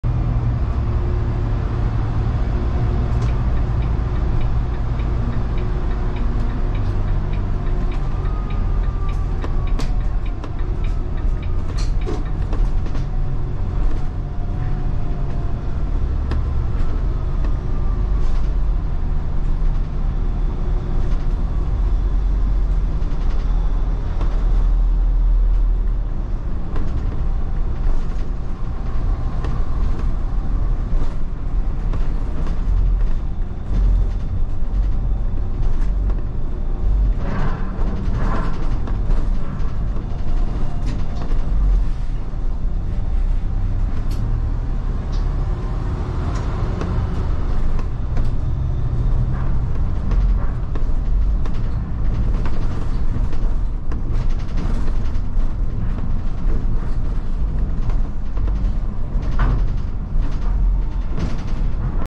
Brisbane City Bus... QSBS
8 AM in the Queen St Bus Station ... Always busy